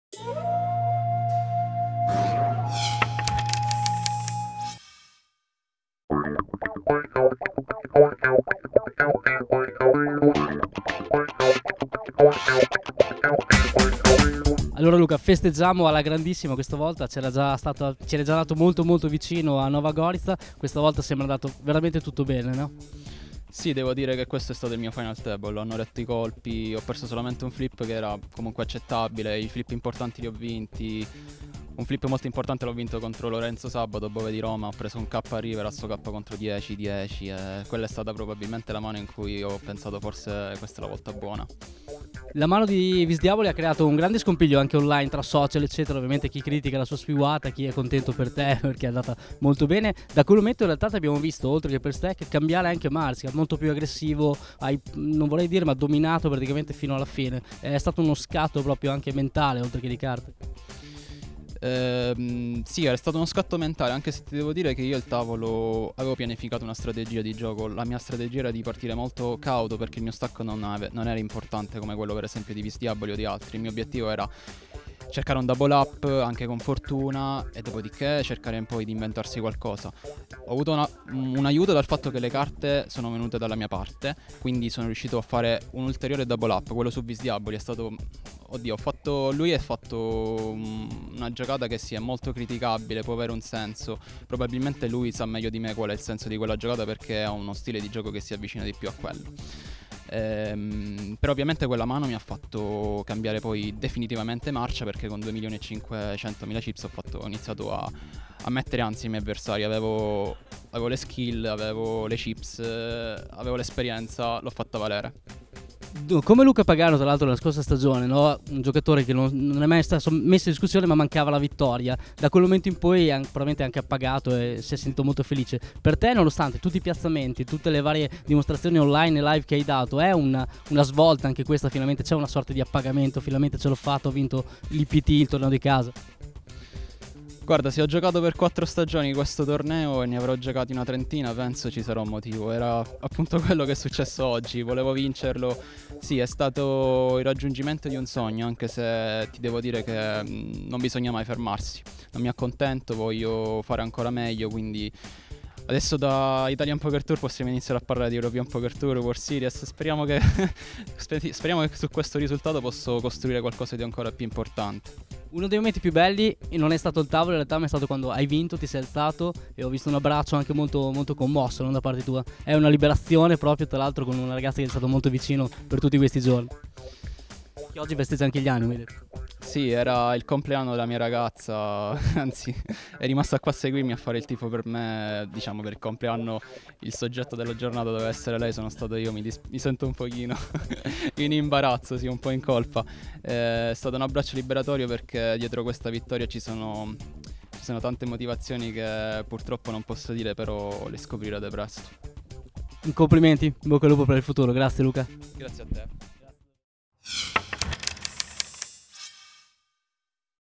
IPT Saint Vincent - Luca Moschitta: le emozioni di una vittoria
Si conclude questo IPT a Saint Vincent, la prima volta del circuito di Stars nella location Valdostana, coincide anche con la prima vittoria di Luca Moschitta che corona così il suo sogno di vittoria. L'abbraccio commosso di fine evento con la sua fidanzata, è una liberazione che da finalmente il via ai meritati festeggiamenti.